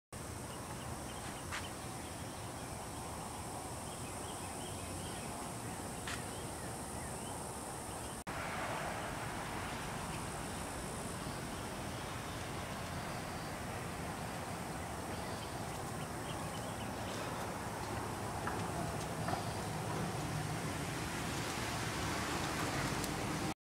Newly Installed Gas Pumps At 7-11 Store
Newly Installed Gas Pumps At 7-11 Store, Port Orange, Florida near Interstate 95